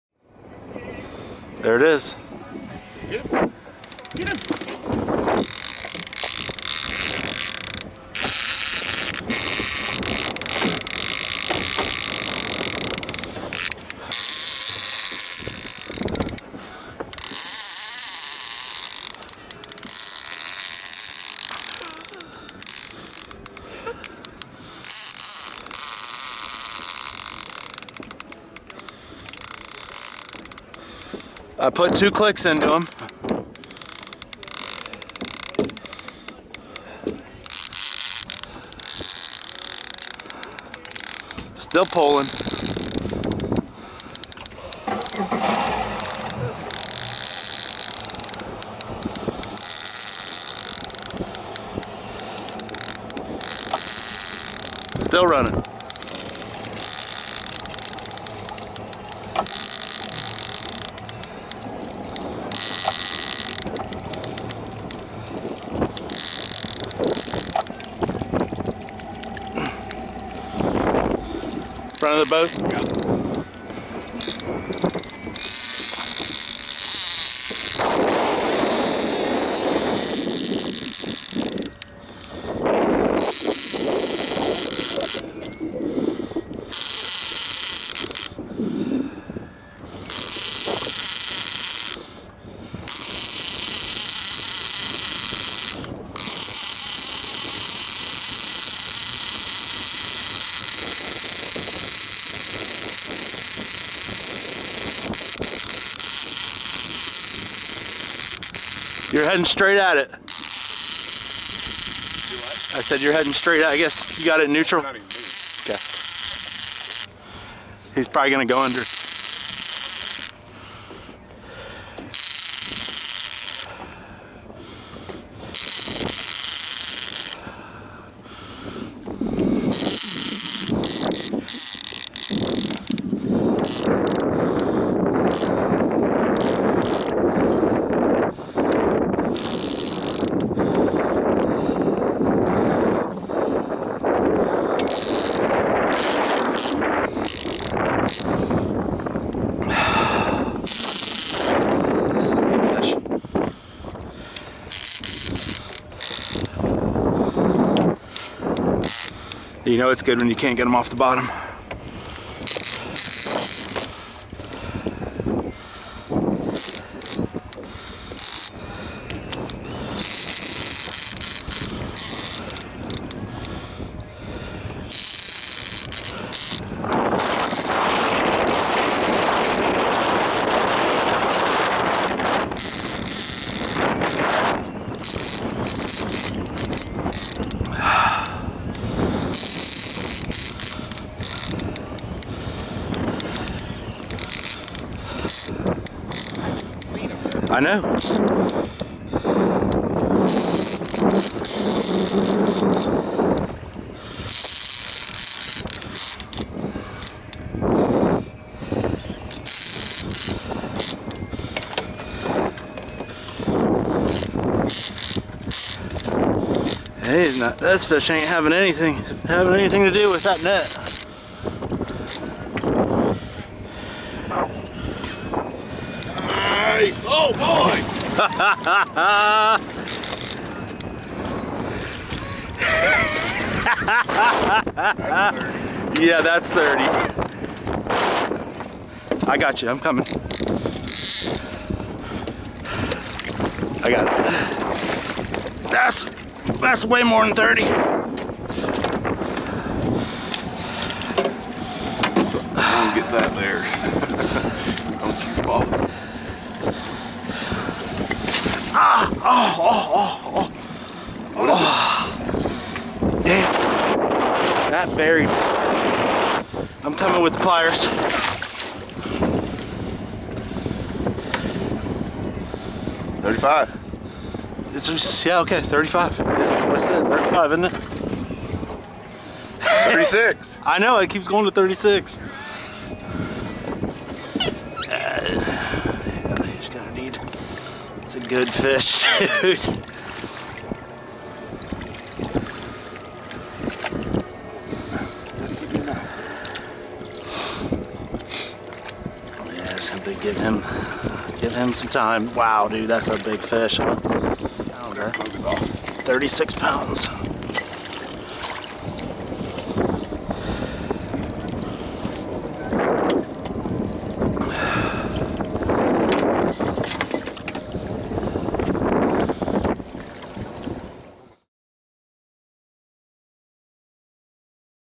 Big Striped Bass Puts on a Show.
We had a full spread of planer boards out and little no action so we brought Three of the Four Planer Boards in. Simon flet it was a good idea to leave One on far outside to run the along the bank. He was right. This fish put on a great show. If you notice I nearly had the hook buried into to my finger. 36# Striper Caught & Released.